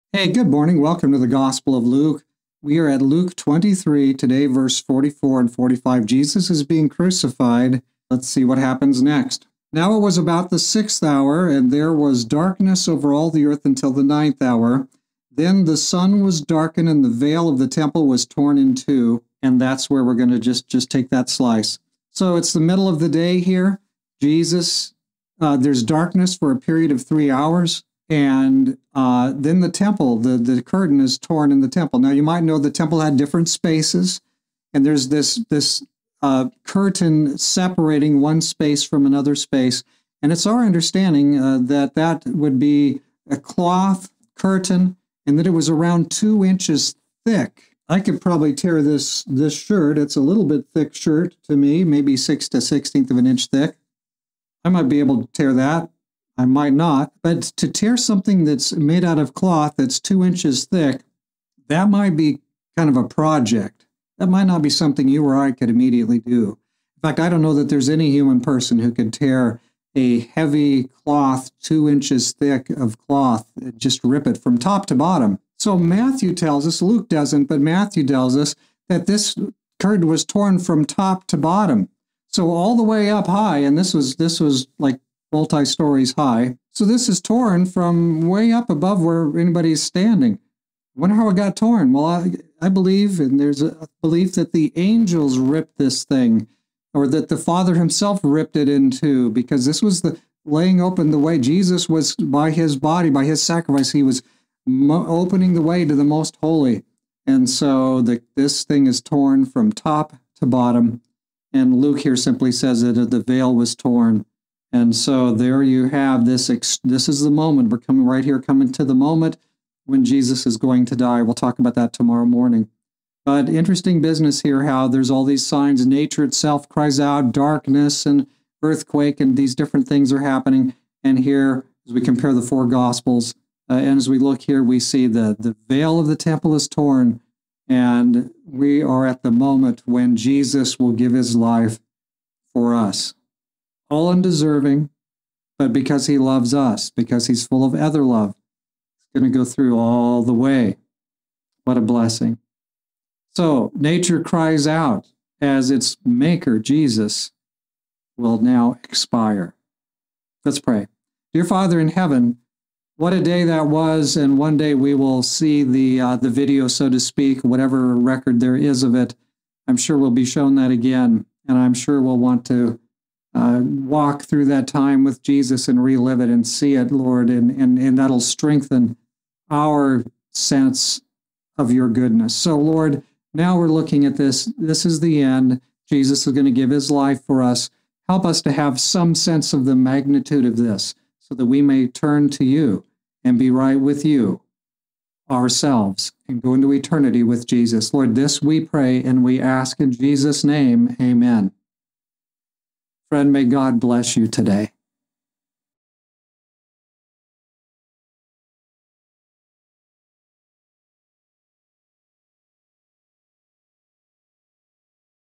Luke 23:44-45 Darkness & Torn Veil
As Jesus hangs upon the cross darkness covers the earth and finally the veil of the temple is torn.